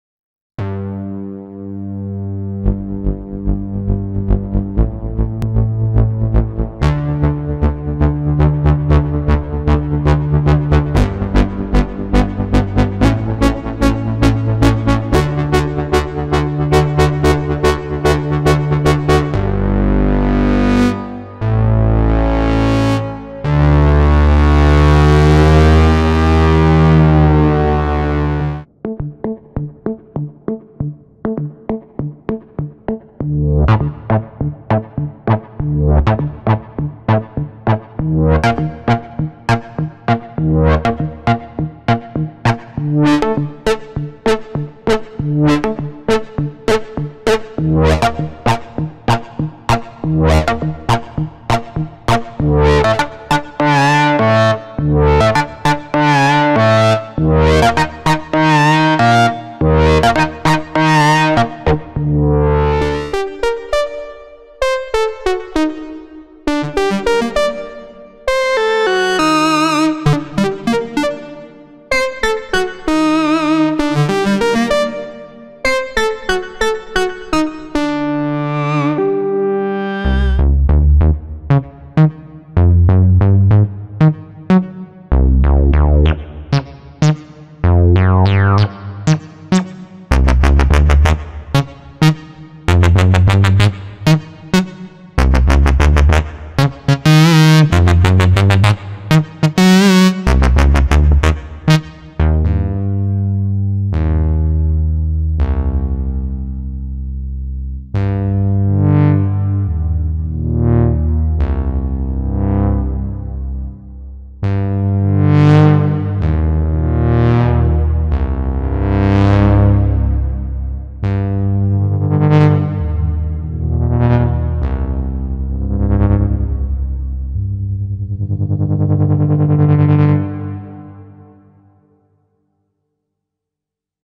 [Expressive E] Touché & Moog Sub 37
Touché & Moog Sub 37
Expressive E의 독특한 익스프레션 컨트롤러인 Touché는 외장 하드웨어와 연결하여 하드웨어 신디사이저의 표현력을 높여주고 사운드의 한계를 뛰어넘을 수 있게 해줍니다.
Moog Sub 37에 대한 프리셋을 Touché에 저장하여 사용할 수 있으며 컴퓨터 없이도 스탠드얼론으로 구동되는 Touché의 모습을 영상으로 확인해보세요

00:00 Touché & Moog Sub 37  Sunny Lead
00:27 Touché & Moog Sub 37  Percuss Dub
01:02 Touché & Moog Sub 37  Little Lyric
01:19 Touché & Moog Sub 37  Funk Bass
01:41 Touché & Moog Sub 37  5th In Line